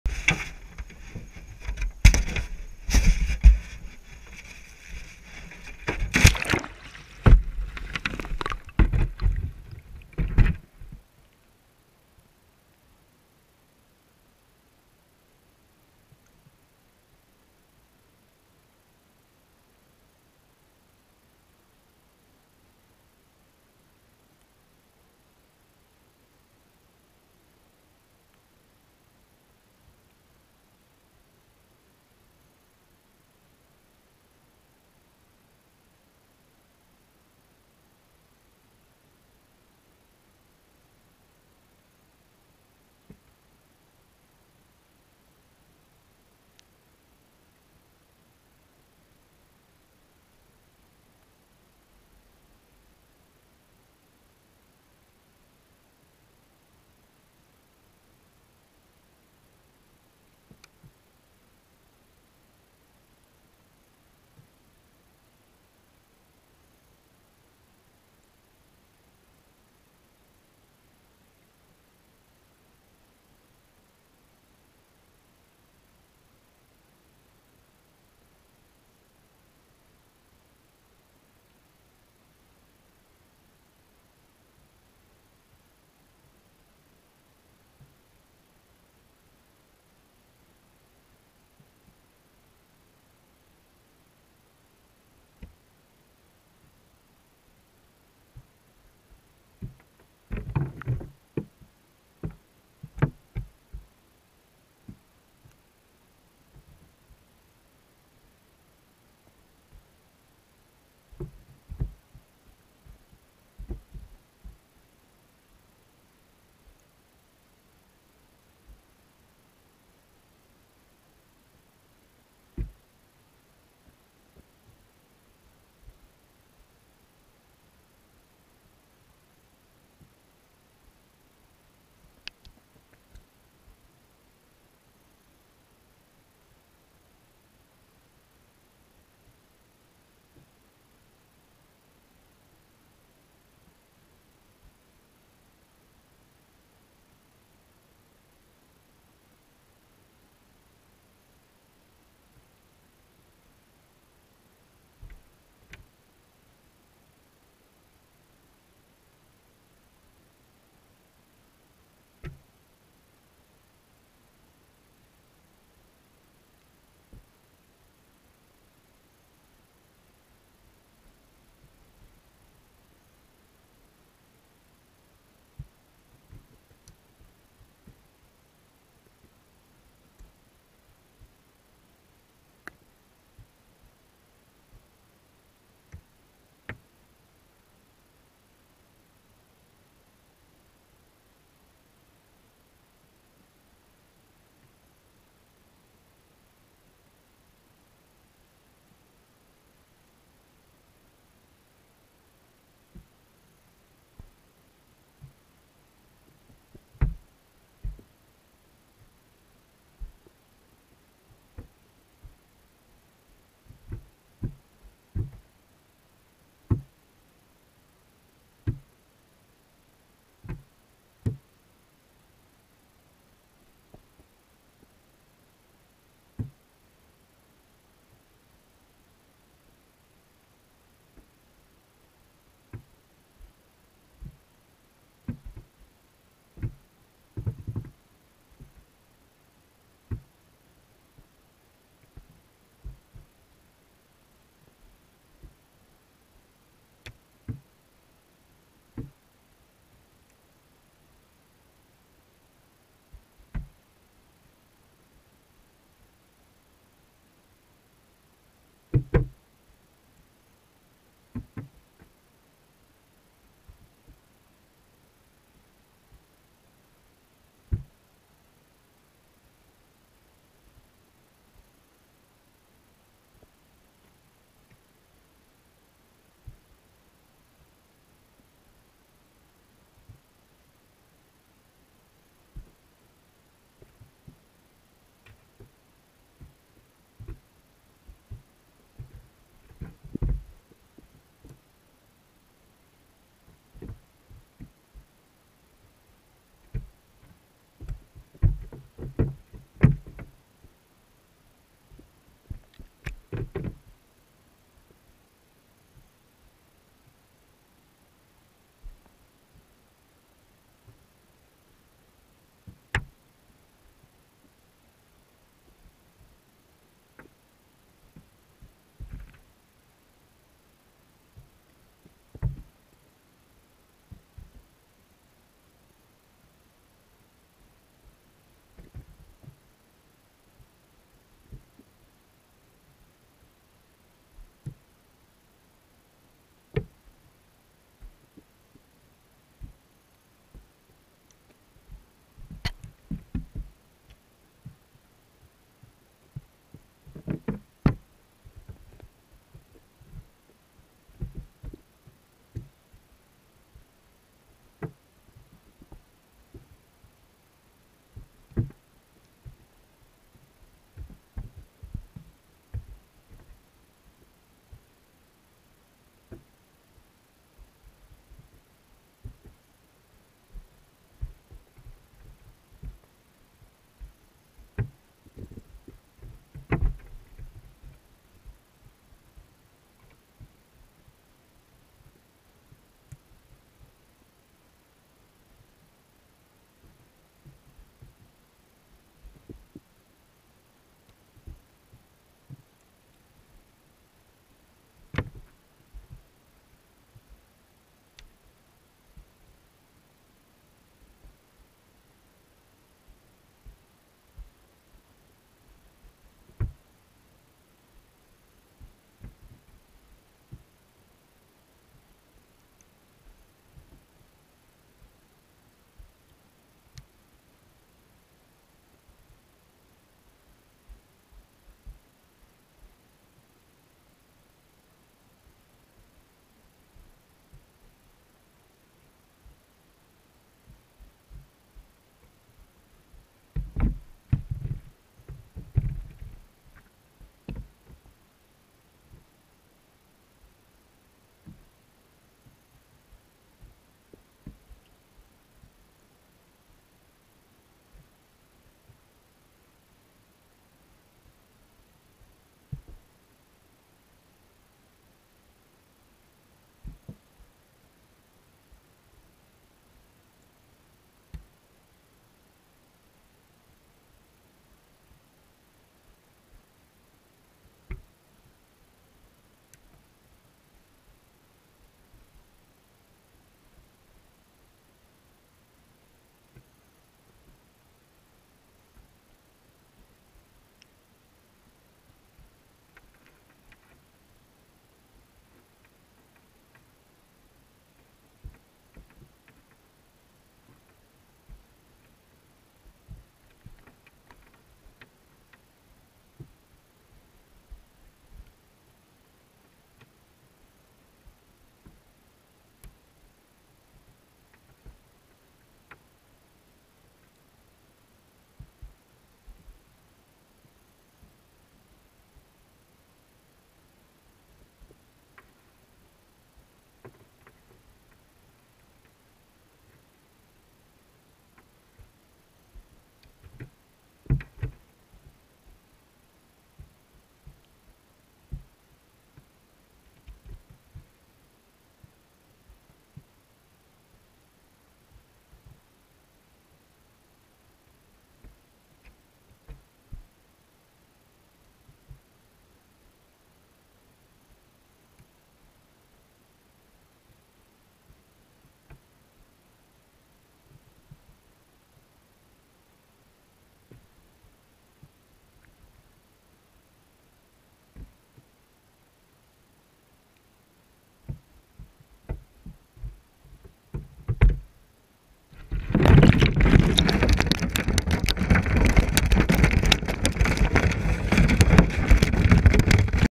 Catching Fish from a Minnow Trap - POV
This is the point of view from a minnow trap in a small stream in Kentucky. The fish caught were mostly rock bass and some bluegill. The bait used was a piece of bread and a small piece of chicken. All the fish were released after the filming of this video. I used the GoPro Hero3 to film this.